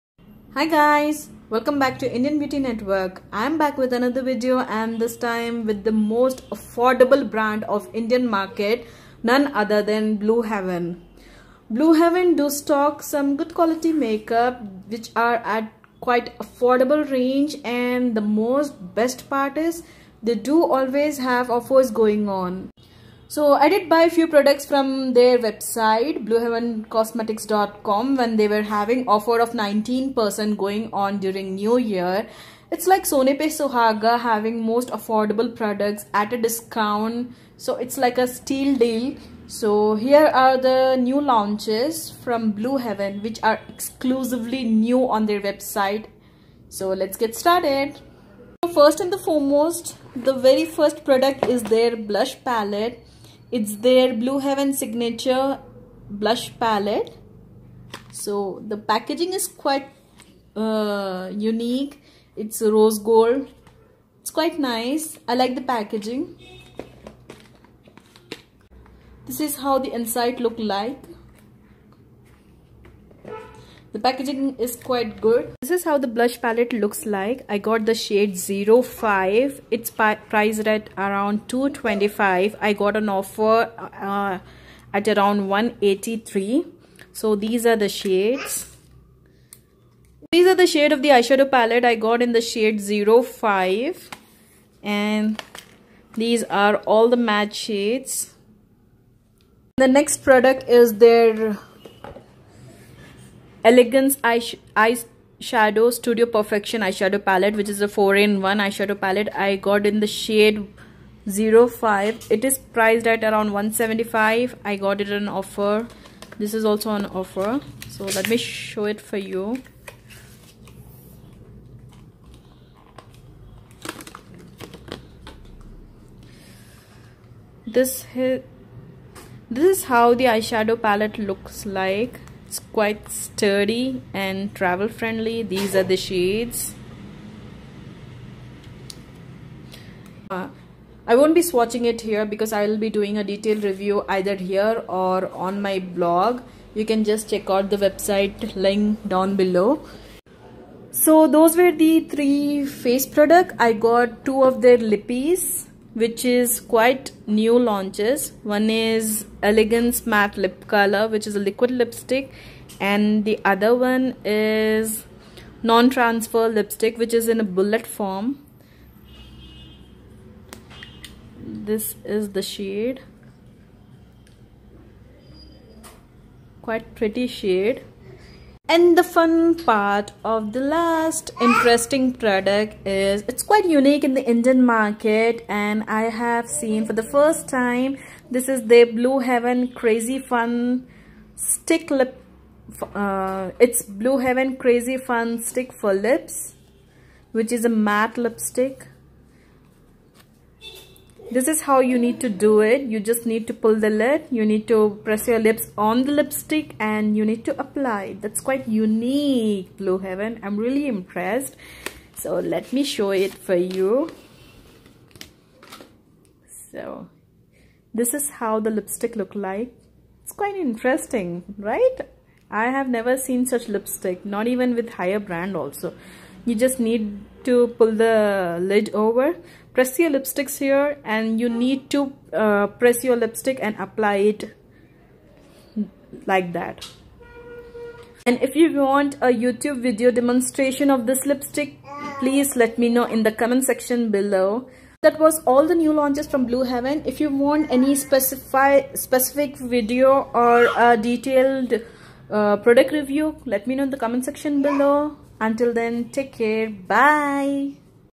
Blue Heaven New Launches|Affordable Makeup under Rs. 500!
Hi Girls!

   I am back with budget series where will be talking about affordable makeup products under Rs.500 from Blue Heaven Cosmetics.